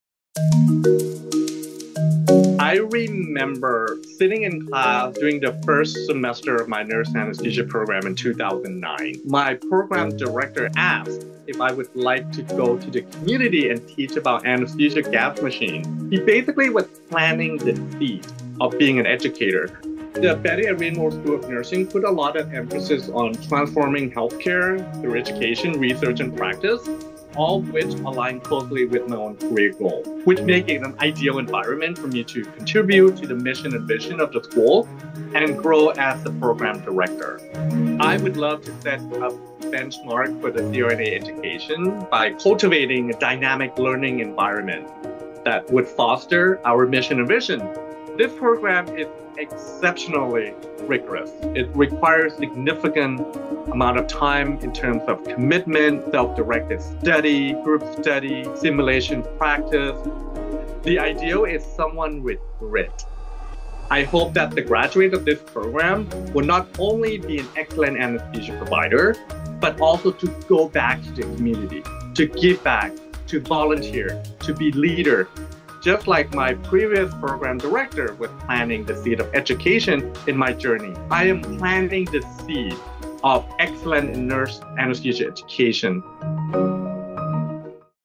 Director of new UC Davis DNP-NA program shares vision
Jakkarin “Jake” Sareerak is the program director for the new Doctor of Nursing Practice — Nurse Anesthesia (DNP-NA) Degree Program at the Betty Irene Moore School of Nursing at UC Davis. He shares his experience as a CRNA and his vision for the program in development.